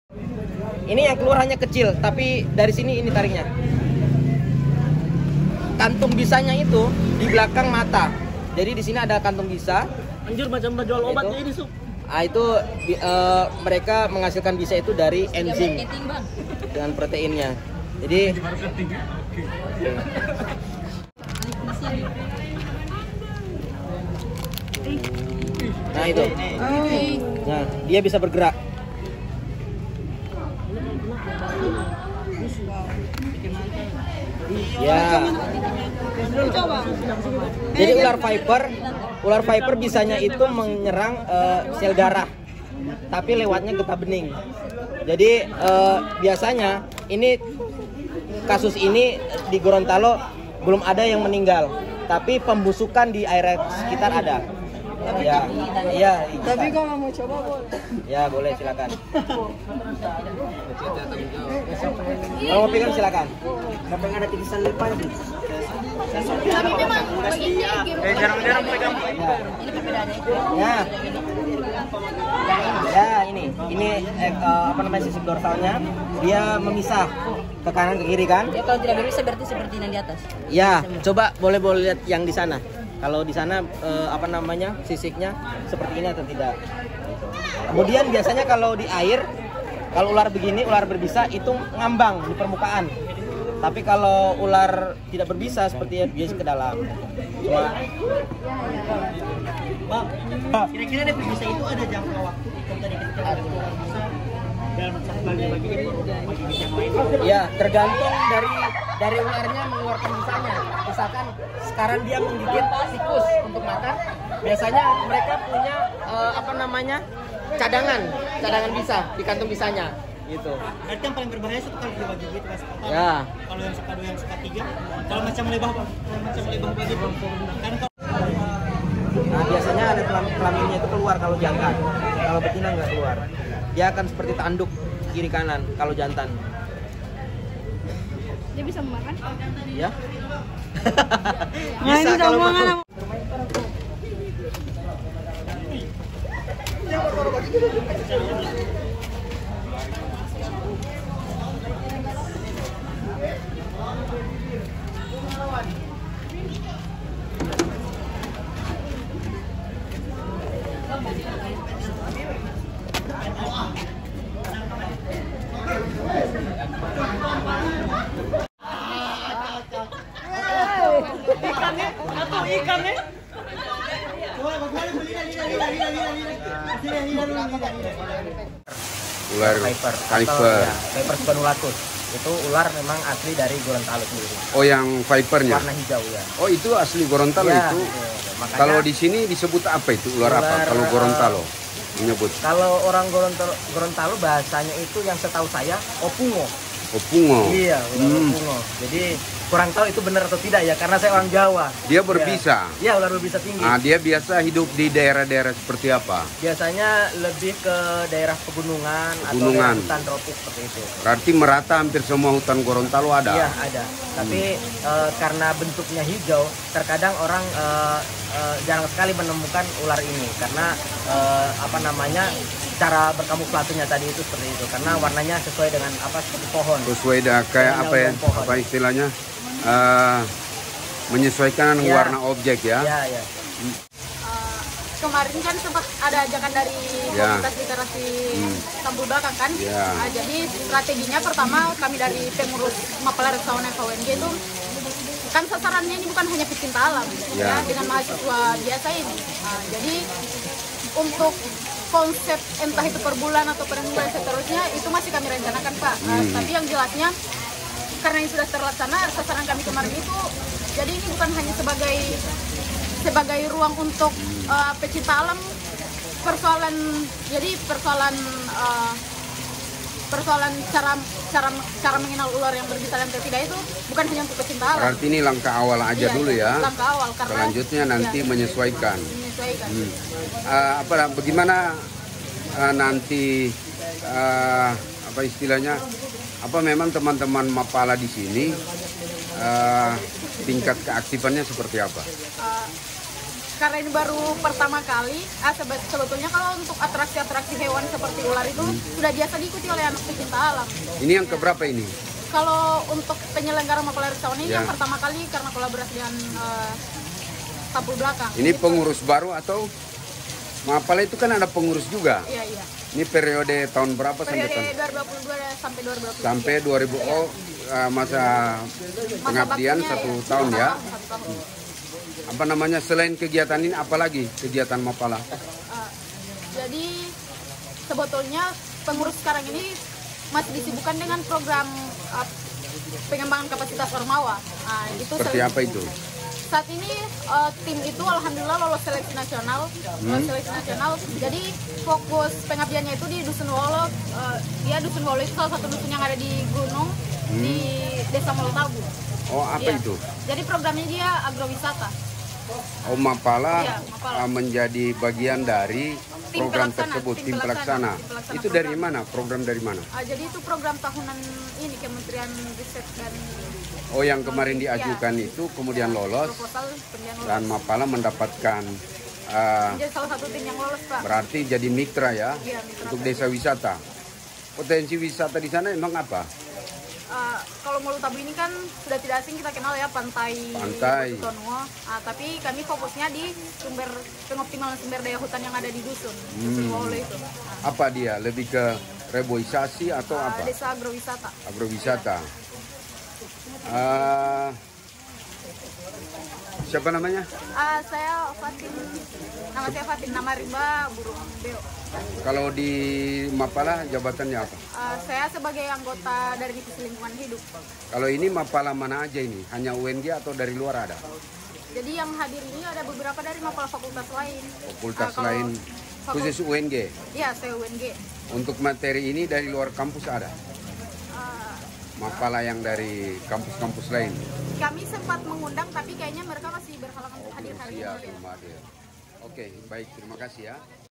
Bisa Mematikan, Waspada !!! Ketemu VIPER Subulatus / ULAR HIJAU EKOR MERAH, Ini Tips nya
Banyak orang menjadi korban dengan gigitan berbisa ular VIPER Subulatus / Ukar Hijau Ekor Merah Ini.

Berbeda dengan ular Piton binatang melata ini karakternya berenang di atas air, dan sering berkamuflase atau bunglon sehingga sering tak diketahui keberadaannya ada didekat kita.

Untuk itu kenali ciri- cirinya melalui video ini oleh komunitas pecinta satwa khusunya ukar dan Mapala UNG memberikan sedikit informasinya.